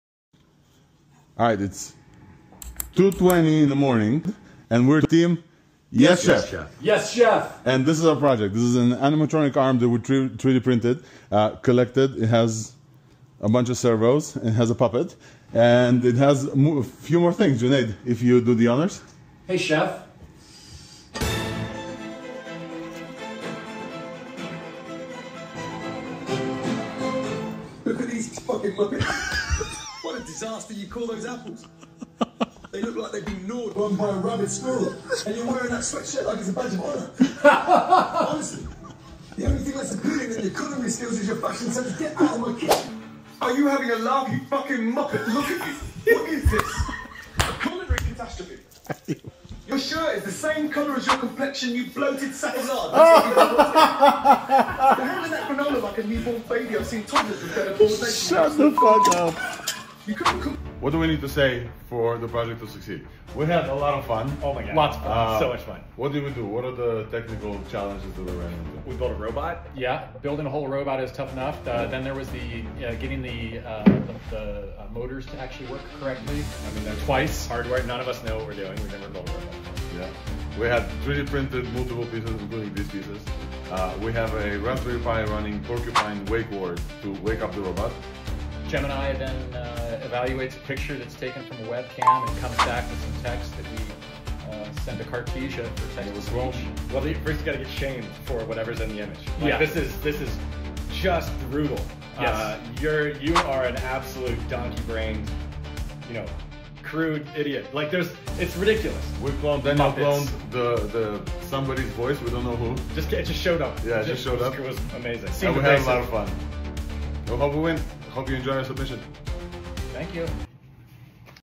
Team Yes Chef hackathon submission video - trimmed length
ROBOT HARDWARE:
Although we 3D printed our parts in advance, our Open Source SO-ARM100 v1.2 was entirely built from scratch during the hackathon. We also attached a random puppet to the gripper to bring it to life.

SERVER:
We setup a Raspberry Pi to run our Yes Chef! server which listens for wake commands, collects video inputs moves the animatron.

INPUT: AUDIO
Microphone from a Webcam, highlighting a wake word and used for clues related to the insults generated.

INPUT: VISUAL
Snapshot from a webcam, passed to Gemini to understand the scene, used as fuel for the muppet’s insults

OBSERVABILITY
Weave. Genuinely amazing with the multimodal aspects. See related link for all the things.

TTS:
Identified text from multimodal inputs sent to Gemini to generate highly curated insults in realtime. Developed a super totally unique voice that helped to bring our chef to life.

Generated background music using Suno because chef loves drama.

ROBOT ANIMATION:
We enabled our robot to “talk” by mapping audio signals to robot movements through our own code to interact with the robot, heavily based on what we learned from and/or what was available in LeRobot.